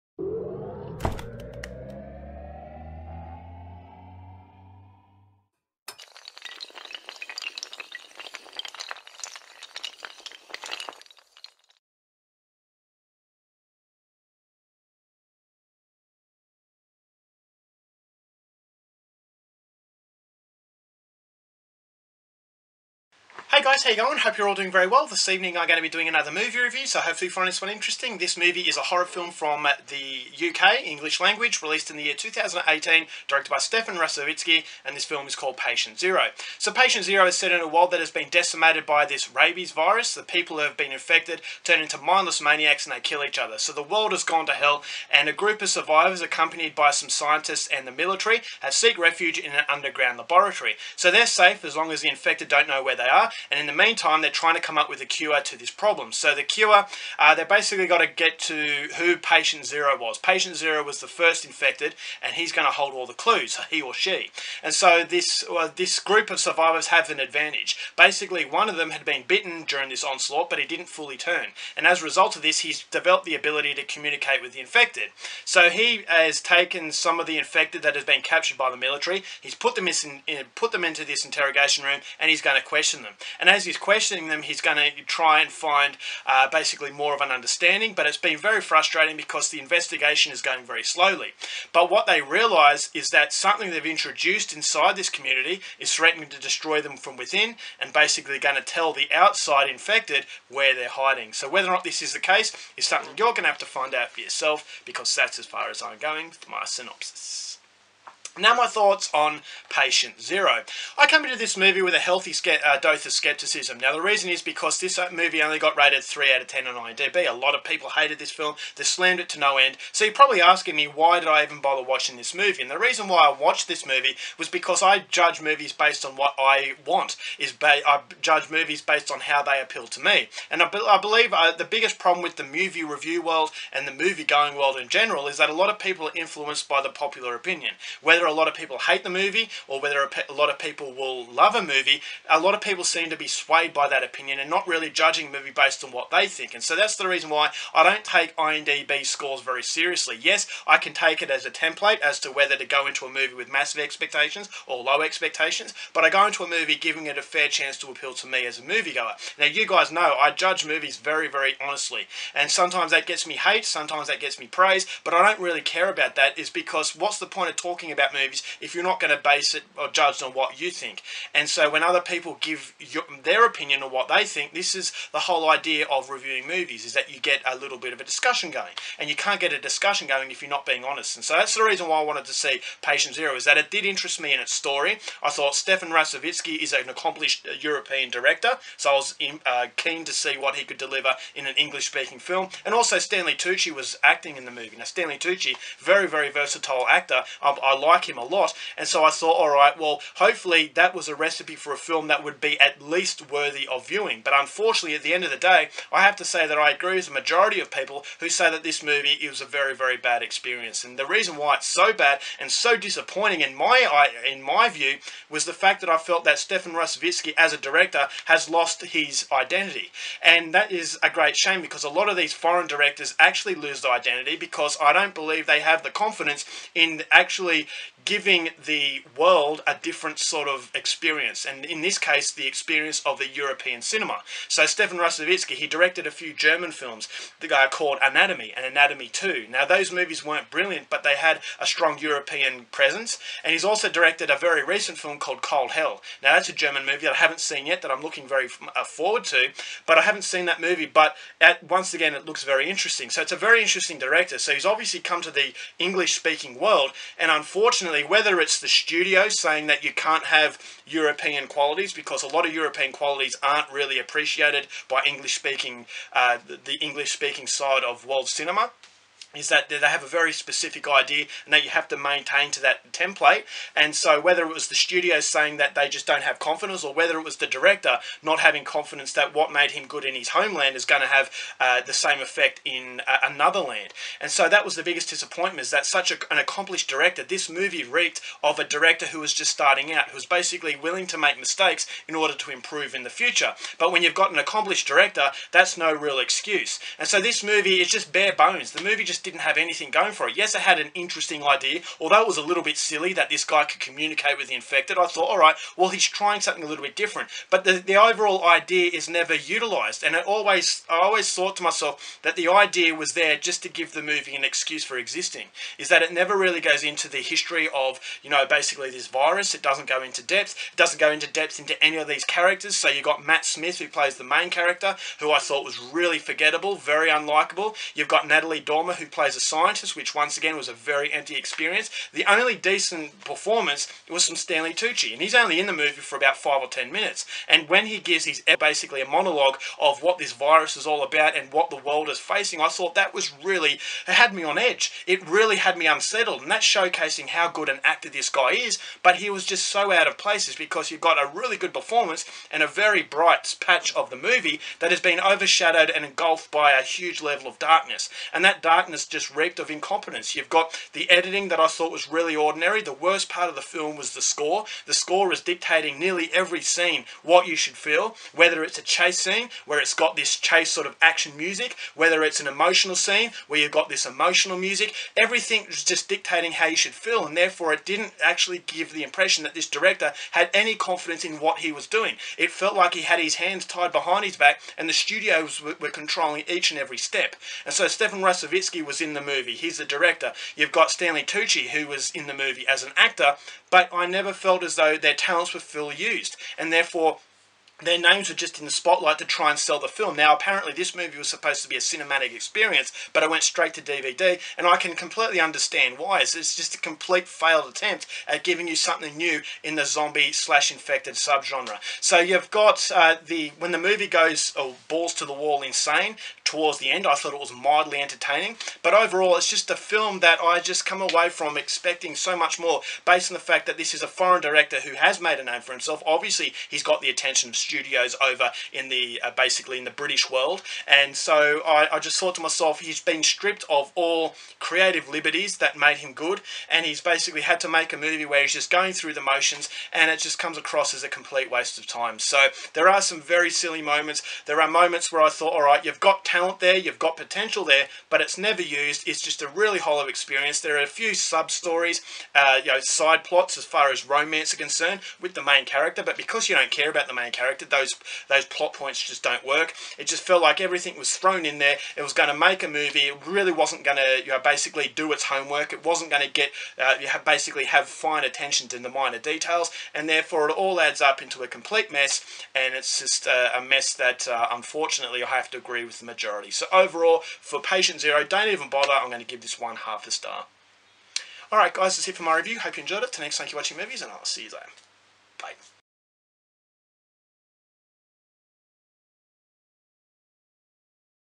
PATIENT ZERO (2018) Stanley Tucci Horror Movie Review
Patient Zero (2018) Movie Review

My review of the Zombie movie, Patient Zero!